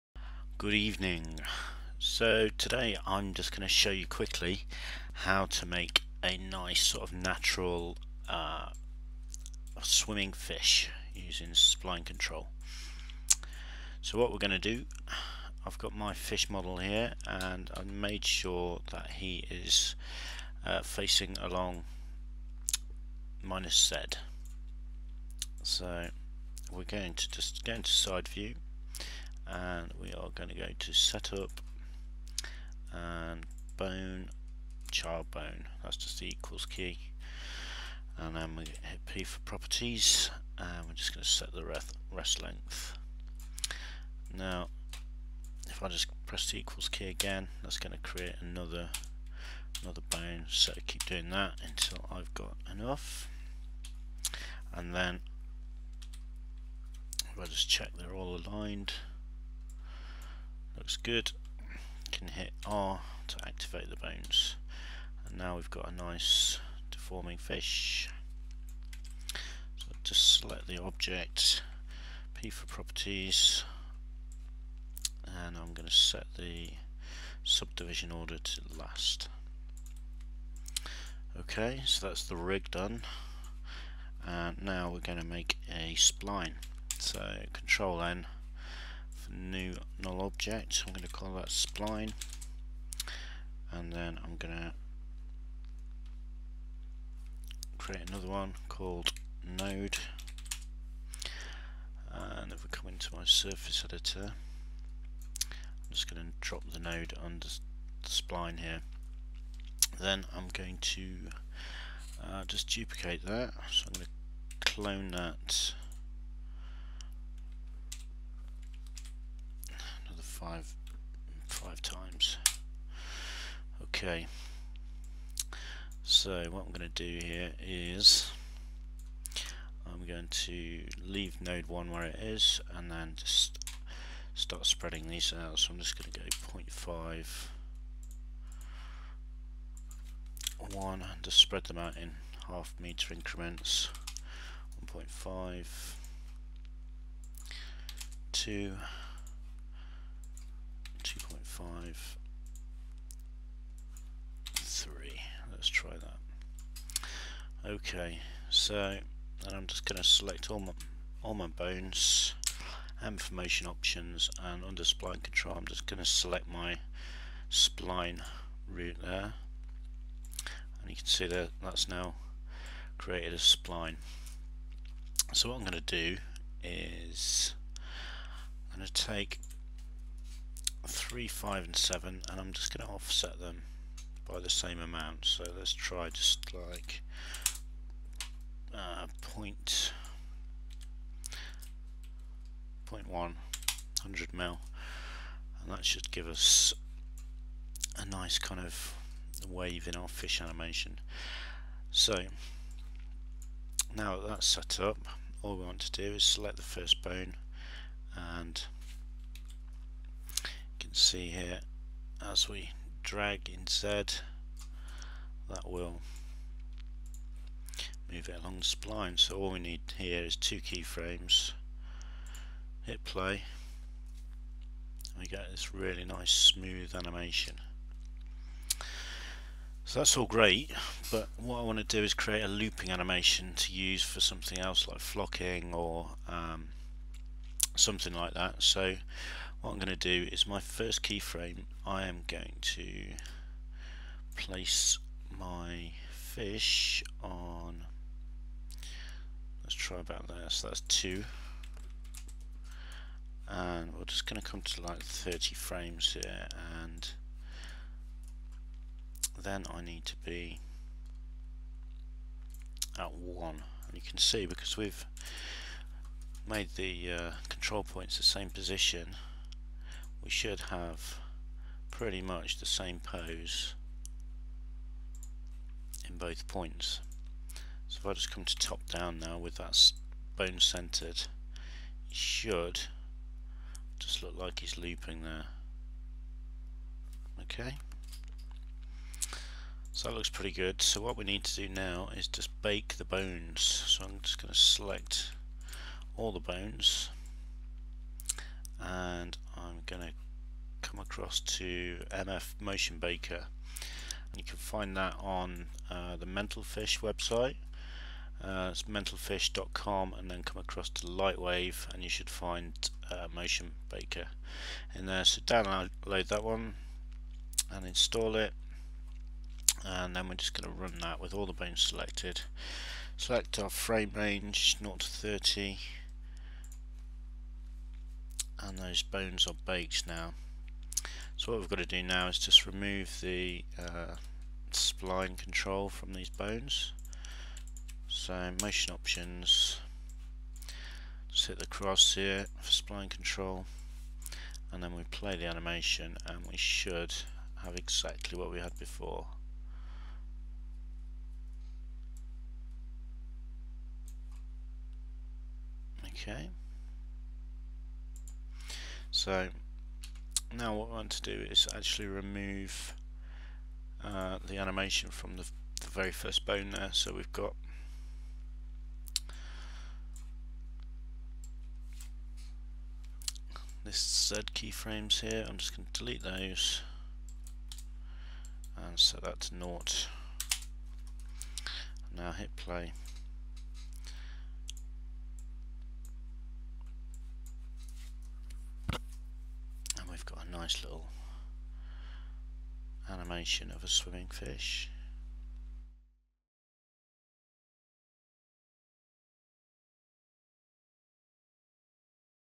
Tutorial: Creating a looping fish animation in LightWave 3D
Quick demo of how I create a looping fish animation using Lightwave 3D's 'Spline Control'.